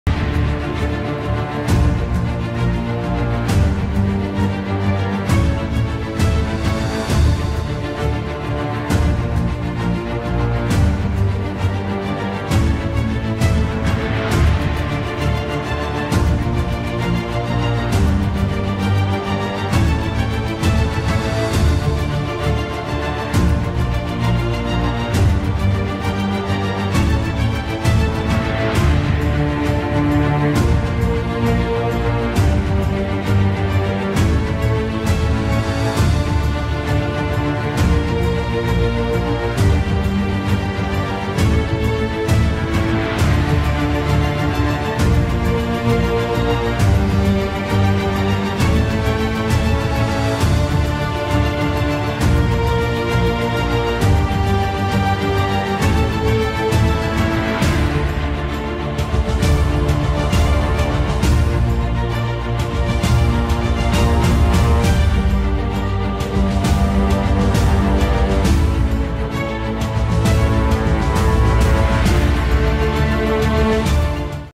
Planes de Gobierno en Girardot
Sabías que la gestión MUNICIPAL, está basada en diferentes Planes de Gobierno a través de los cuales vamos dando las repuestas del 1x10 del Buen Gobierno en las áreas de servicios públicos y social, en este video te mostramos cuales son.